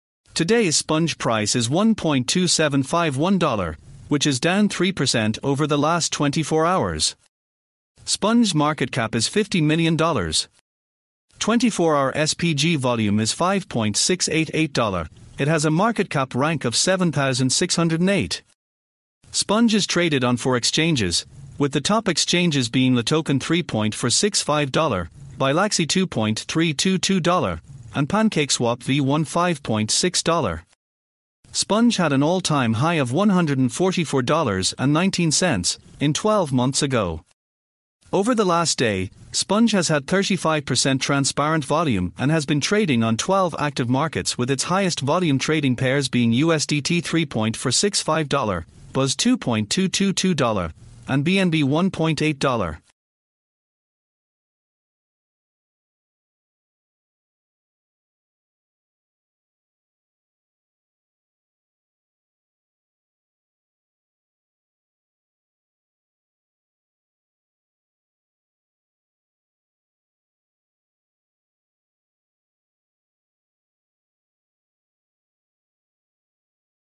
SPONGE COIN - SPG SPONGE CRYPTO PRICE PREDICTION - WHERE TO BUY & FUTURE PRICE ?
Synopsis: Today's Sponge price is 1.2751 $, which is down 3% over the last 24 hours. Sponge's market cap is 50 million $. 24 hour SPG volume is 5.688 $, It has a market cap rank of 7608. Sponge is traded on 4 exchanges, with the top exchanges being LATOKEN 3.465 $, Bilaxy 2.322 $, and PancakeSwap V1 5.6 $. Sponge had an all time high of 144.19 $, in 12 months ago. Over the last day, Sponge has had 35% transparent volume and has been trading on 12 active markets with its highest volume trading pairs being USDT 3.465 $, BUSD 2.222 $, and BNB 1.8 $